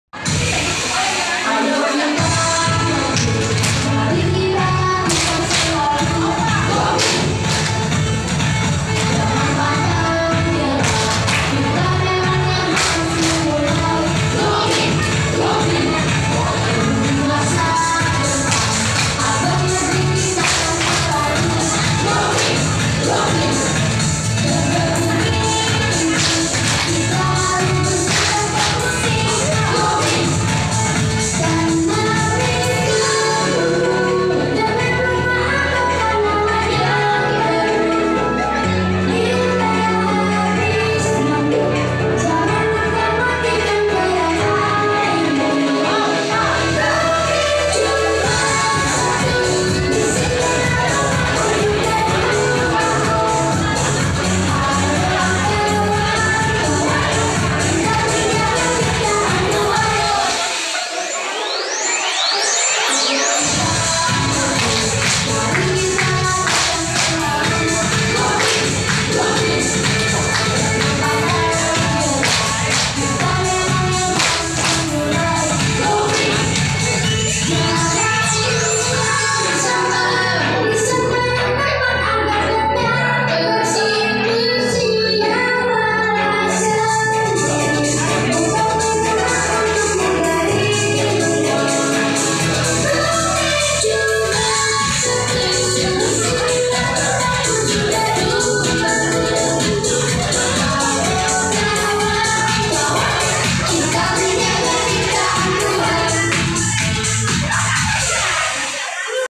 Senam go green guru paud kreatif# indriaza
Bersama guru2 paud dari brebes, tasikmalaya, cirebon, subang, cianjur, kuningan, bandung